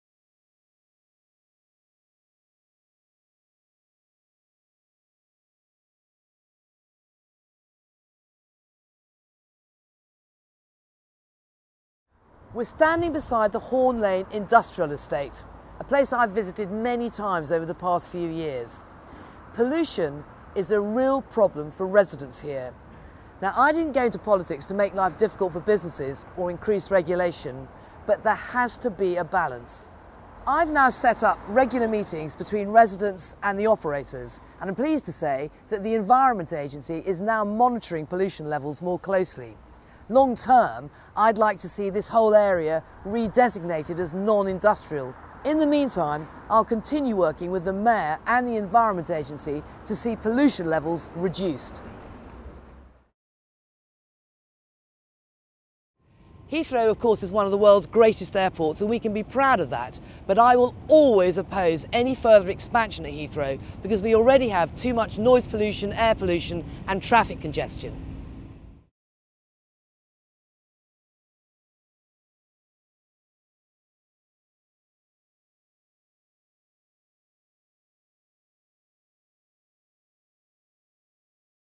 Angie Bray on the environment and Heathrow
Angie gives her views on Ealing and Acton's environment, air pollution and Heathrow expansion.